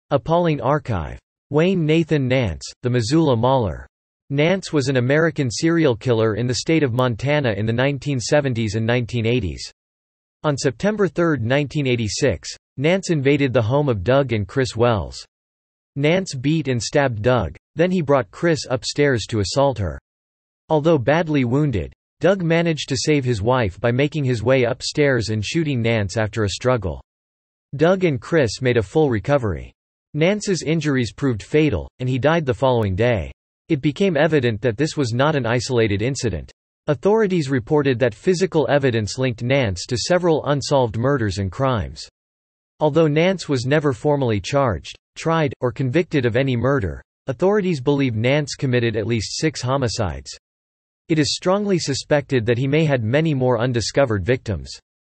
Wayne Nathan Nance: The Missoula Mauler
Wayne Nance was born on October 18, 1955, in Clinton, Montana to George Edwin Nance (August 20, 1928 – April 4, 2004), a long-haul truck driver, and Charlene Mae Nance née Mackie (February 2, 1936 – April 3, 1980), a waitress. Nance lived in a motor home outside of Milltown, Montana, which is east of Missoula and was described by teachers and classmates as an academically-gifted yet eccentric individual who was also a juvenile delinquent.  He went on to...